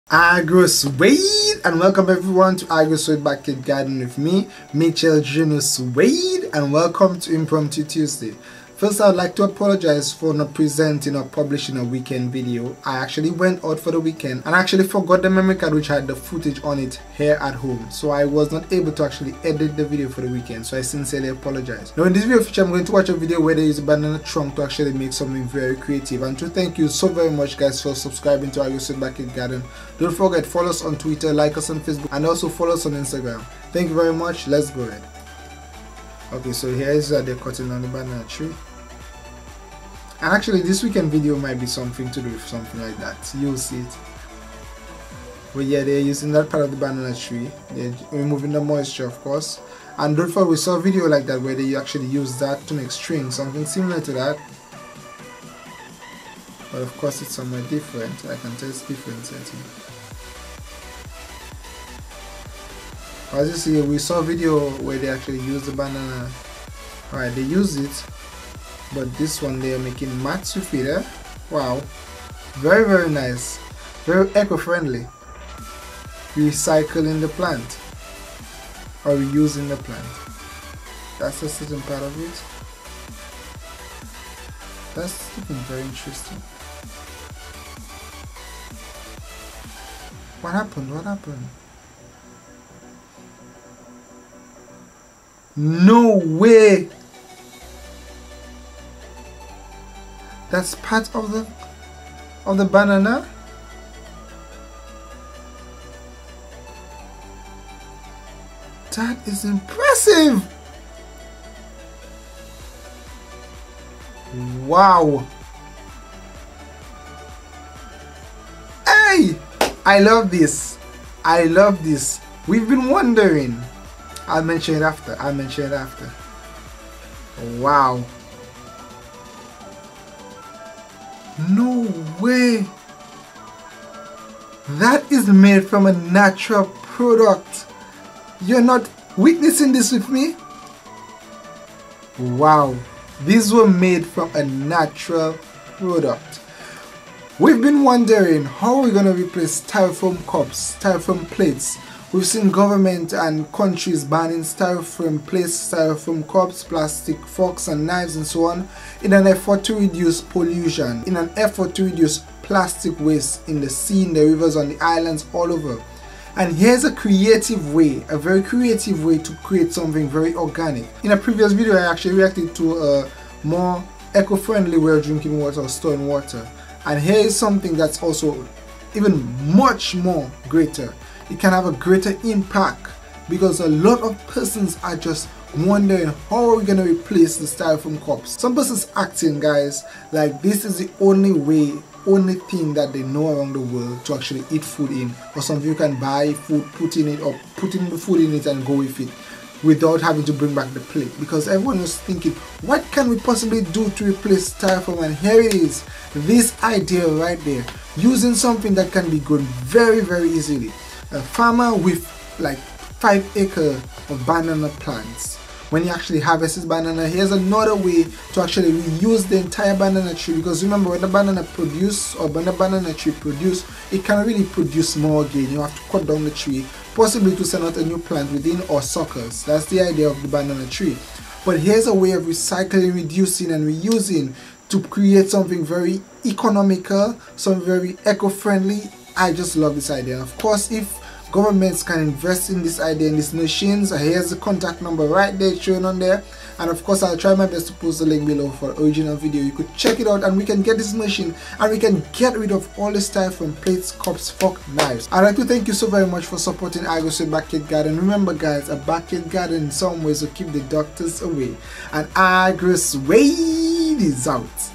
How we can replace Styrofoam plates || Eco friendly plates || Agrosuede ✔
We've always wondered...what can be done to reduce Styrofoam plates pollution around the world!

How about we start by cutting off production of this materials!

How about changing the mindset of these owners of these Styrofoam factories!

How about we use an eco friendly material to create a product which is safe for the environment.

Enjoy this video presentation!